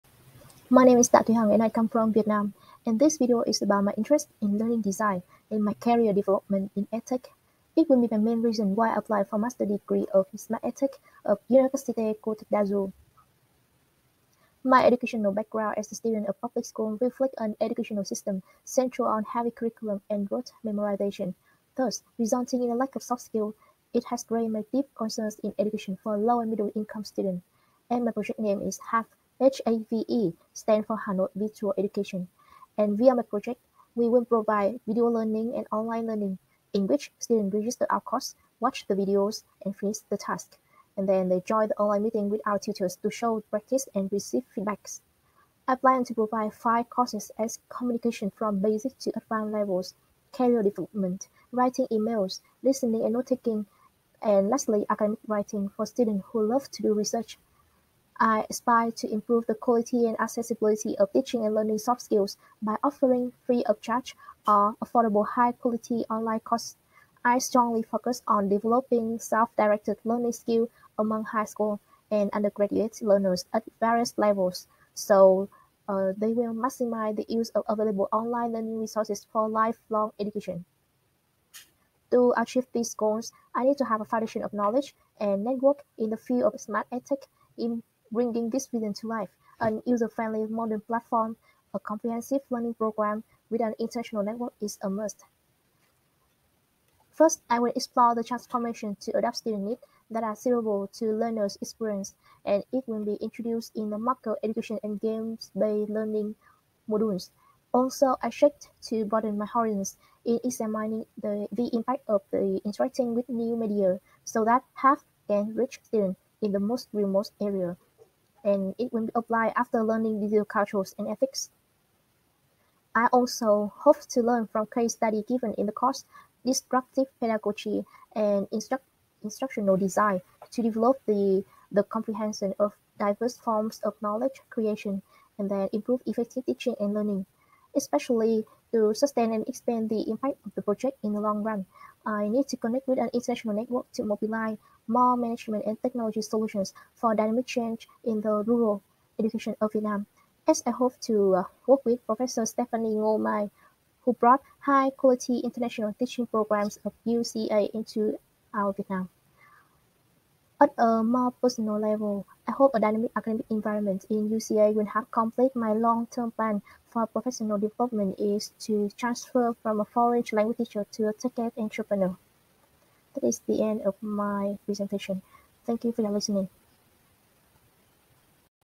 TA THUY HANG_Video motivation_UCA
This video is expressing my desire to enroll in MSc SmartEdTech program, the project to be developed and the ideas for career development following the program.